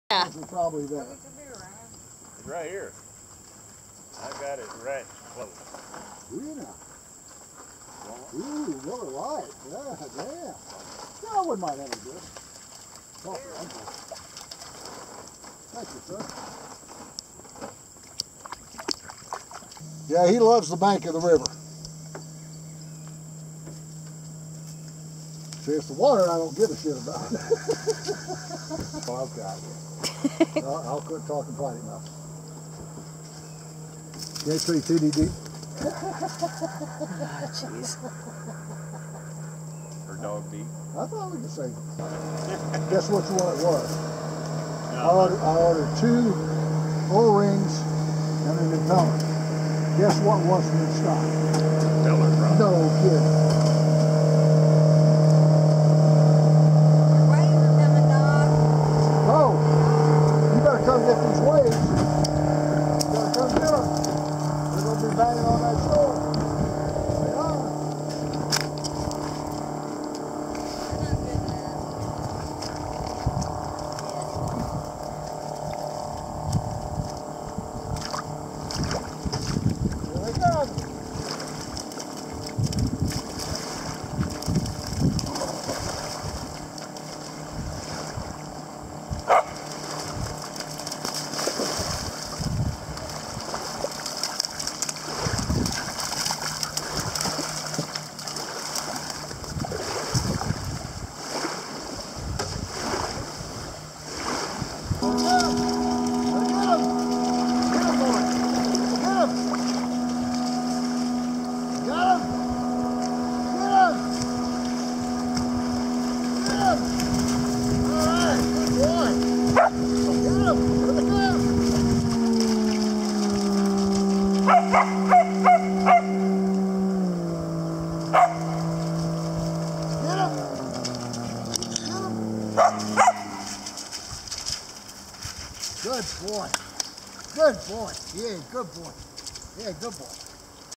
Border Collie
The boundless energy of a young border collie as he chases waves on the shore of the Current River.